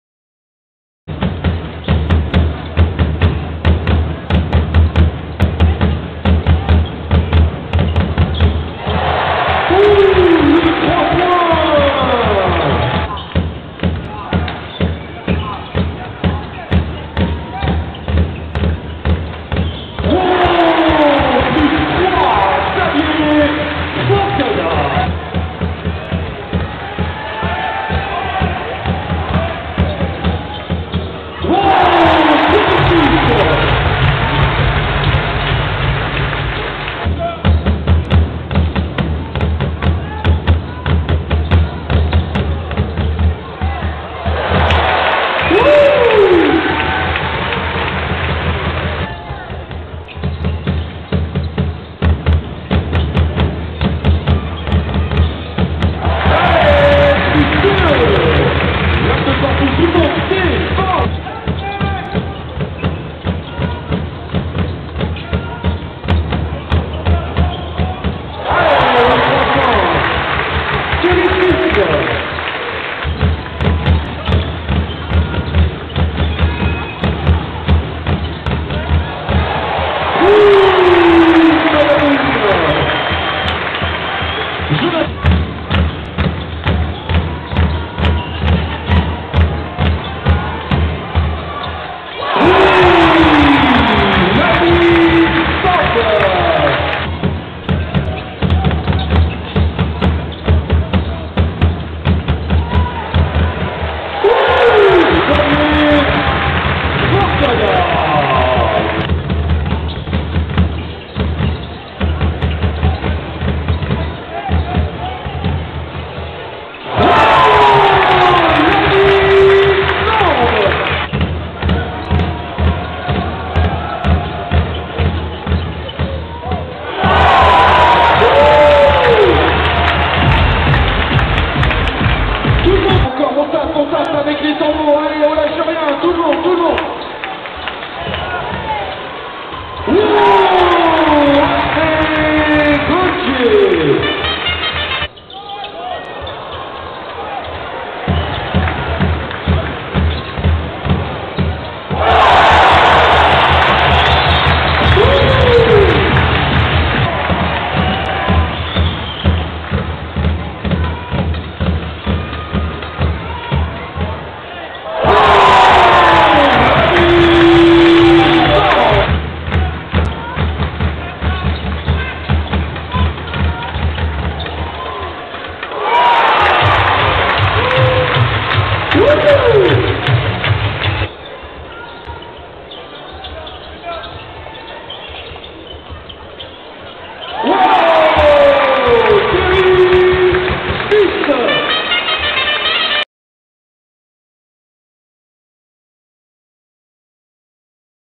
Highlights | NBH / Elan Chalon (86/100)
Les meilleurs mouvements du NBH lors de la rencontre de vendredi soir face à l'Elan Chalon.